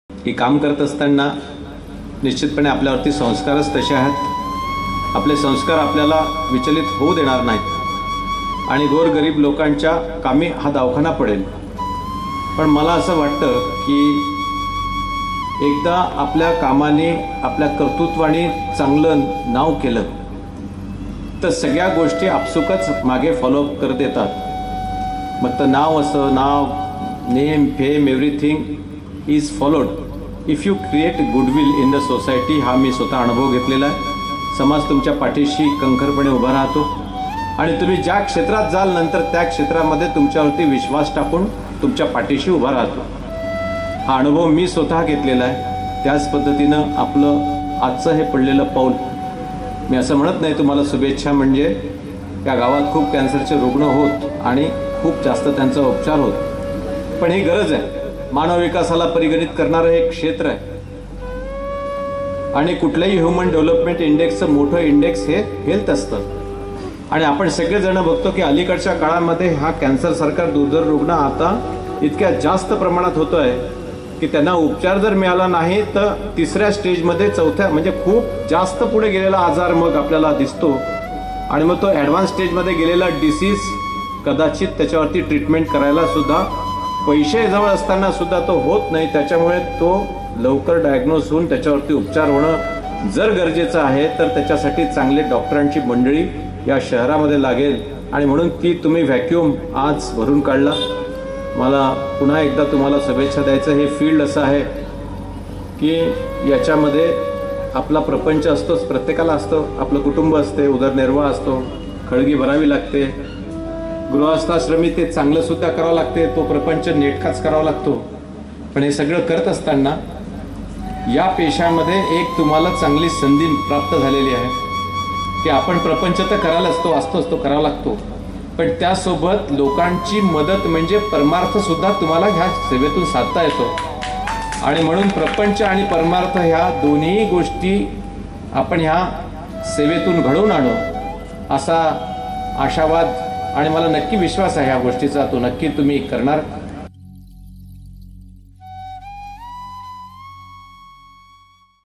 अकोला ! कँसर असेलेल्या व्यक्तीला खूप निराशा असते त्याला आशेचा किरण फक्त डॉक्टर असतो! डॉ रणजित पाटील
Dr Ranjit patil